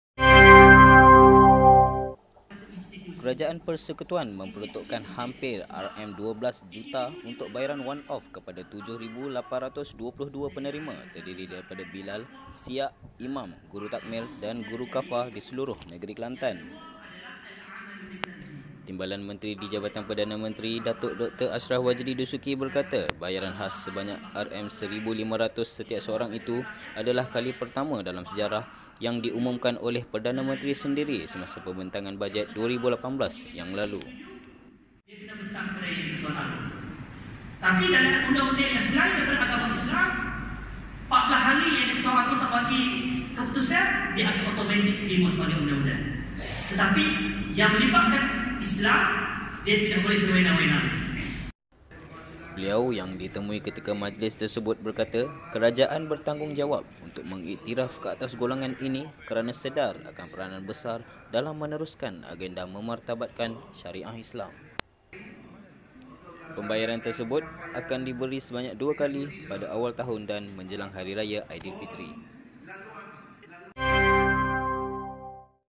RM11.7 juta untuk imam, bilal, siak di Kelantan
KOTA BHARU 13 Feb. - Kerajaan Persekutuan memperuntukkan sebanyak RM 11,733,000 untuk bayaran 'one-off' kepada 7,822 penerima terdiri daripada bilal, siak, imam, guru takmir dan guru Kelas al-Quran dan Fardhu Ain (KAFA) di negeri ini.

Timbalan Menteri di Jabatan Perdana Menteri, Datuk Dr Asyraf Wajdi Dusuki berkata, bayaran khas sebanyak RM1,500 setiap seorang itu adalah kali pertama dalam sejarah dan diumumkan oleh Perdana Menteri, Datuk Seri Najib Tun Razak semasa pembentangan Bajet 2018 yang lalu.